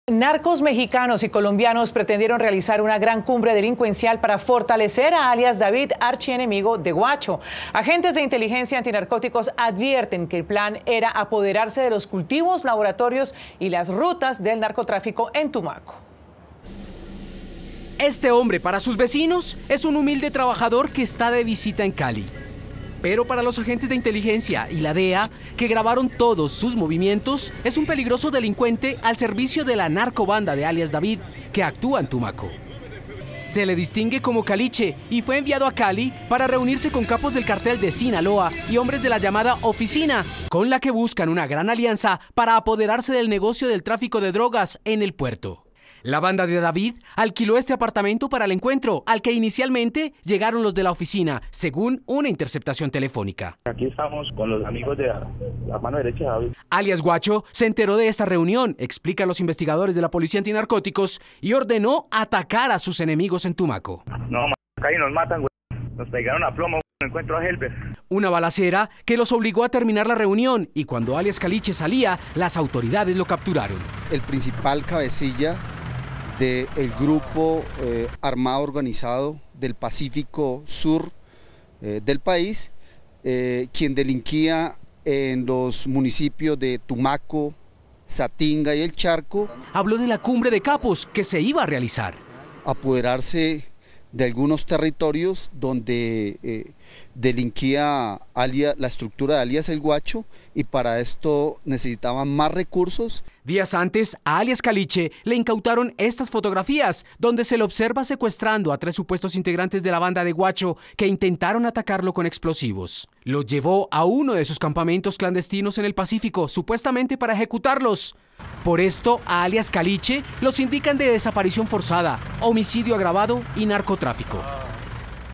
Frustran cumbre de capos del narcotráfico en Cali | Noticias Caracol
Miembros del Cartel de Sinaloa y narcos colombianos buscaban realizar la gran reunión criminal en la capital del Valle del Cauca para fortalecer al archienemigo de alias ‘Guacho’ y tomar el control del tráfico de drogas en Tumaco, Nariño.